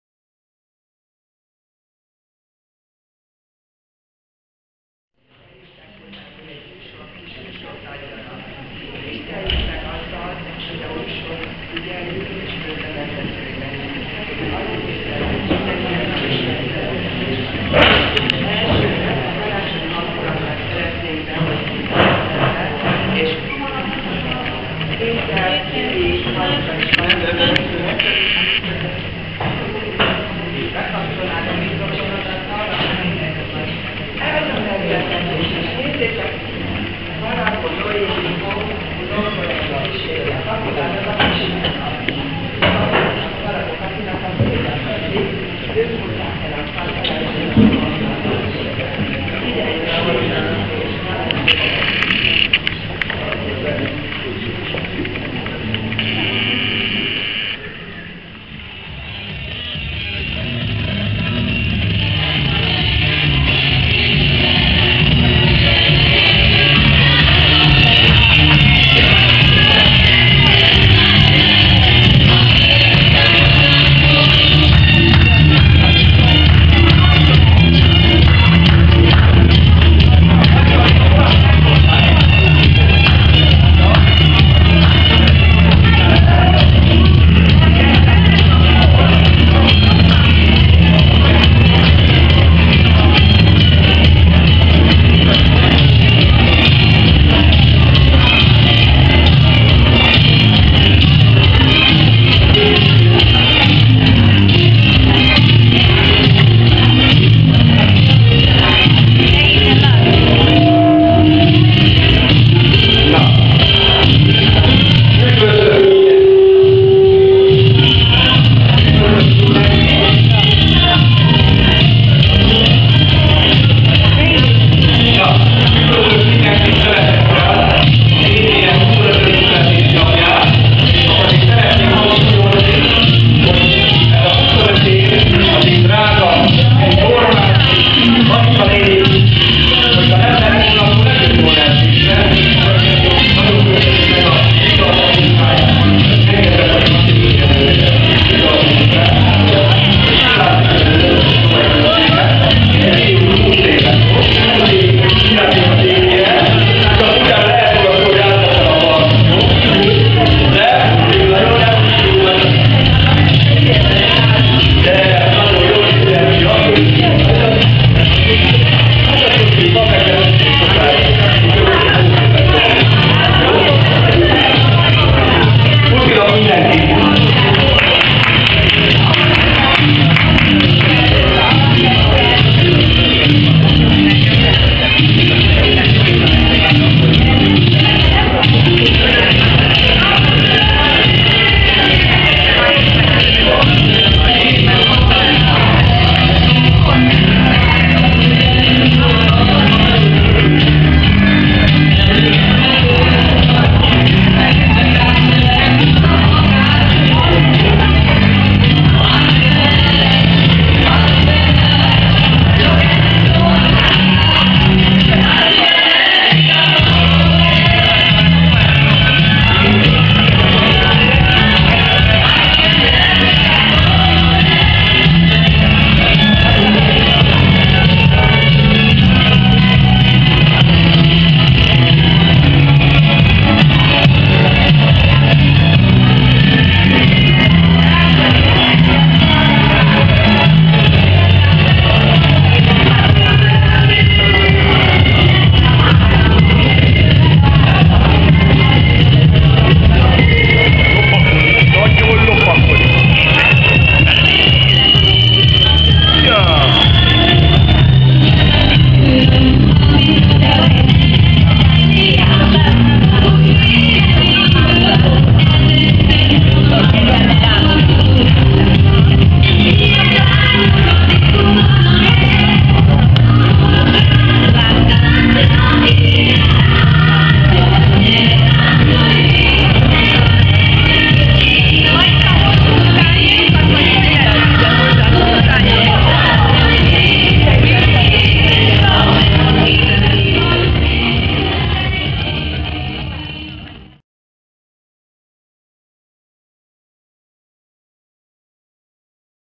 25 D V E Karácsony 2016 12 11 (Hornok Zoltán Videója)
Egyesületünk a Dél-Pesti Vesebetegek Egyesülete December 11.-én ünnepelte a 25. évfordulóját.
25 éve hogy meg alakult az egyesület Tormási Marika vezetésével.
25 éve jóban rosszban szeretetben.